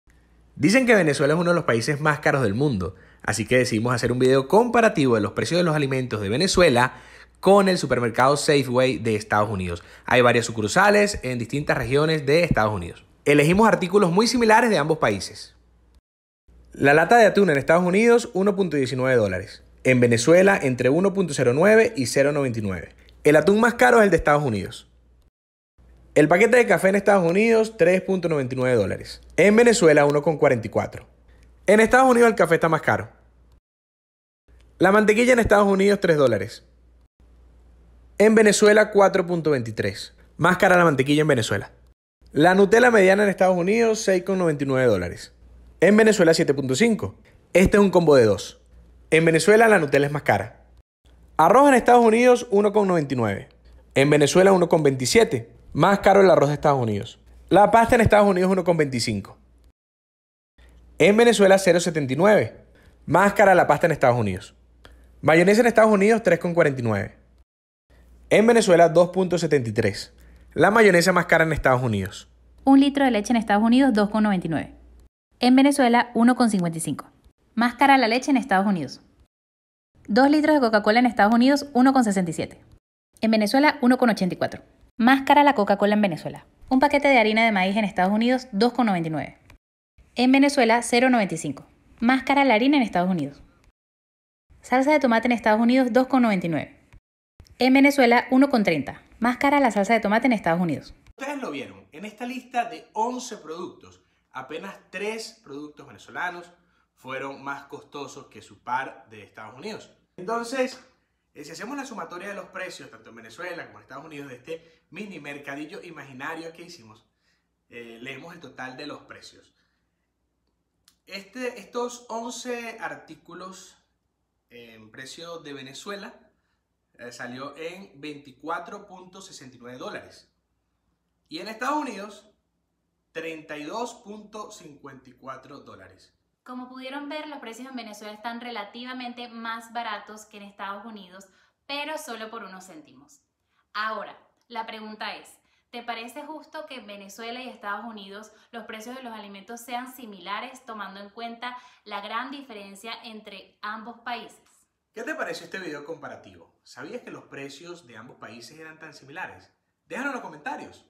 😱¿Venezuela es el país más COSTOSO del Mundo 🌍? | ¿Mito o Realidad?
Hicimos este video comparativo para que descubras si es cierto que los precios en Venezuela están casi iguales que en Estadosunidos 🤯🇻🇪🇺🇸